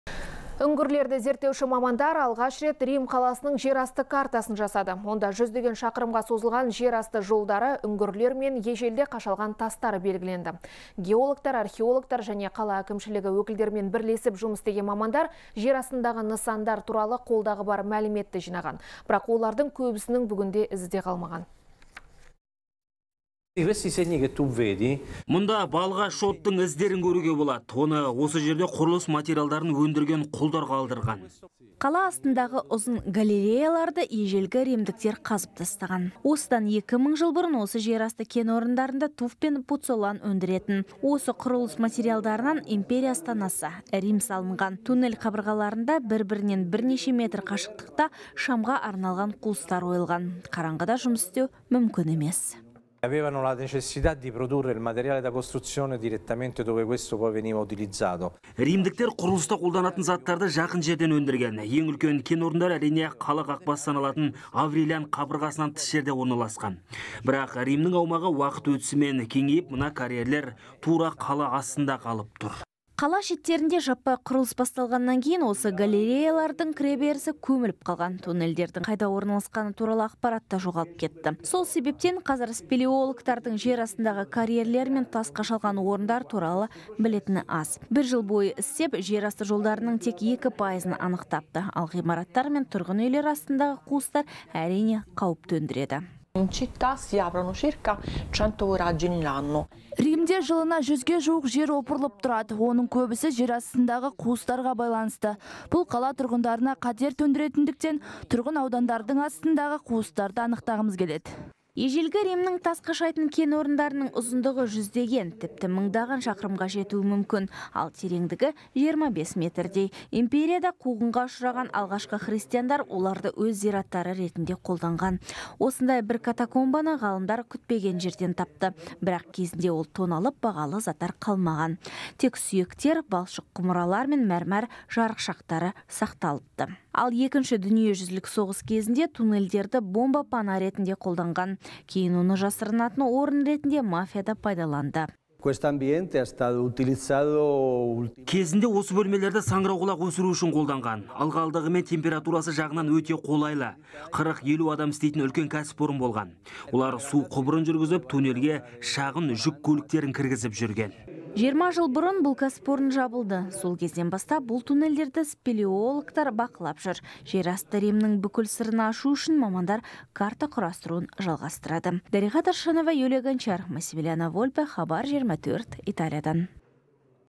Римнің жерасты картасы жасалды
Үңгірлерді зерттеуші мамандар алғаш рет Рим қаласының жерасты қартасын жасады.

Онда жүздеген шақырымға созылған жер асты жолдары, үңгірлер мен ежелде қашалған тастар белгіленді. Геологтар, археологтар және қала әкімшілігі өкілдерімен бірлесіп жұмыс істеген мамандар жерастындағы нысандар туралы қолдағы бар мәліметті жинаған, бірақ олардың көбісінің бүгінде ізі де қалмаған.

Мұнда балға шоттың іздерін көруге болады. Оны осы жерде құрылыс материалдарын өндірген құлдар қалдырған.

Қала астындағы ұзын галереяларды ежелгі римдіктер қазып тастаған. Осыдан екі мың жыл бұрын осы жерасты кен орындарында туф пен пуццолан өндіретін. Осы құрылыс материалдарынан империя астанасы Рим салынған. Туннель қабырғаларында бір-бірінен бірнеше метр қашықтықта шамға арналған қуыстар ойылған. Қараңғыда жұмыс істеу мүмкін емес.